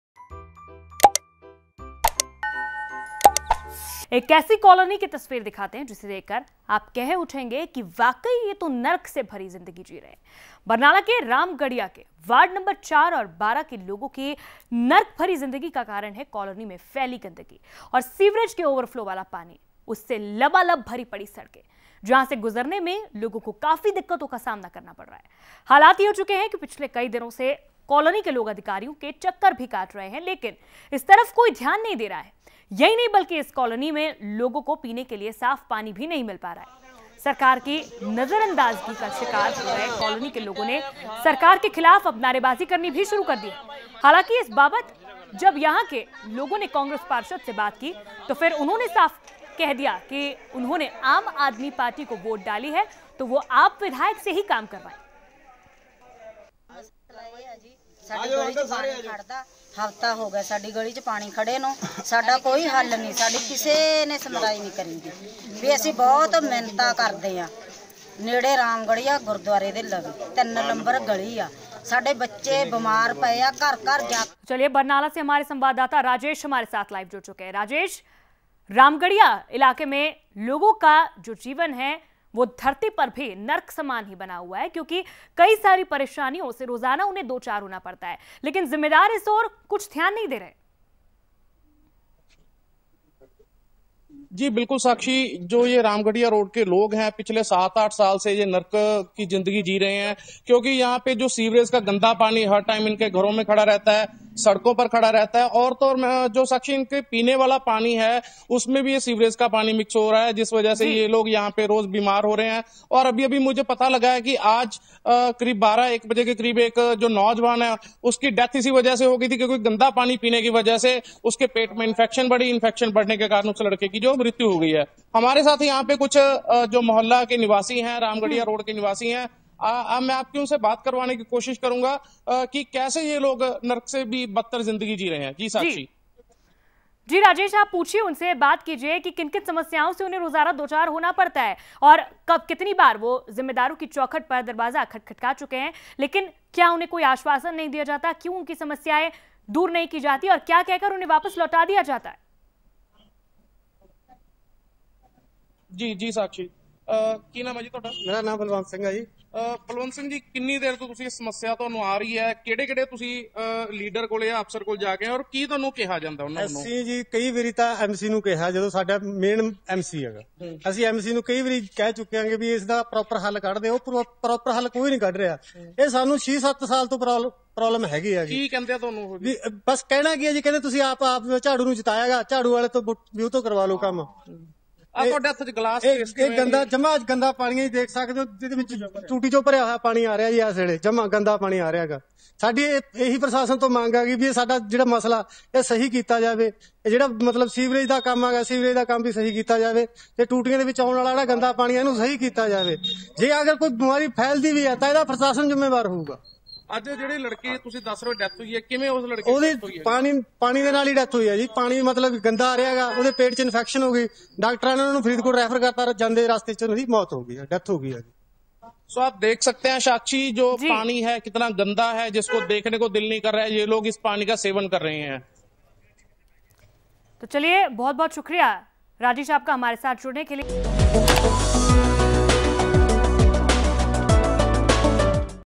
नरक भरी जिंदगी गुजारने को मजबूर लोग, सीवरेज के गंदे पानी में लबालब सड़कें
नरक भरी जिंदगी गुजारने को मजबूर लोग !
रामगढ़िया कालोनी में आओ, तो जरा संभल कर आना
सीवरेज के गंदे पानी में लबालब सड़कें

Fastway:     66
Digi Cable: 195
Hathway:   222
Den:            313